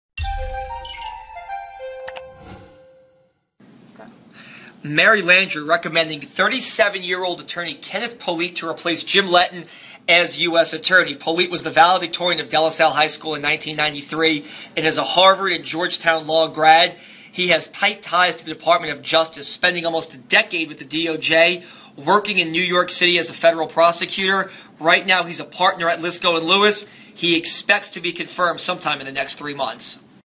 Sen. Landrieu recommends Polite for top fed prosecutor
Sen. Mary Landrieu is recommending private practice attorney Kenneth Polite Jr. for the position of U.S. Attorney in the Eastern District of Louisiana.